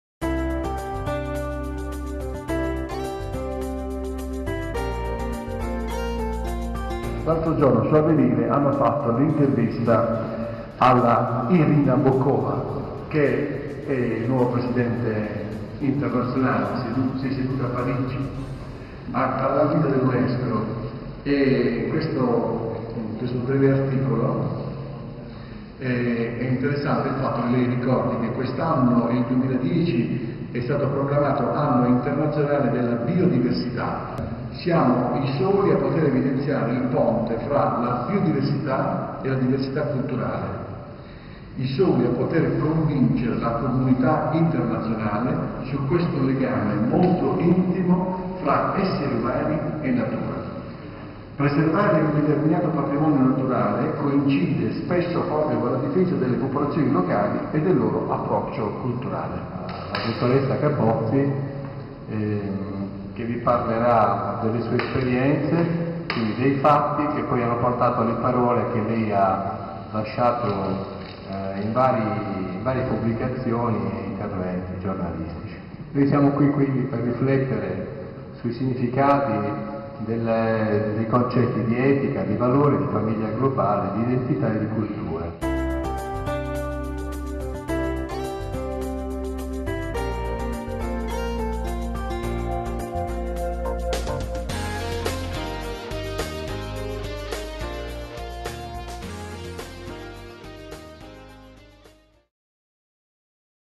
Convegno su famiglia, identità e culture (Club Unesco Bari)
Nel mondo globale in cu viviamo, quali valori e spazi sono rimasti alle famiglie? Roberto Lorusso insieme alla dott.ssa Capozzi ne parla in un convegno organizzato dal club Unesco di Bari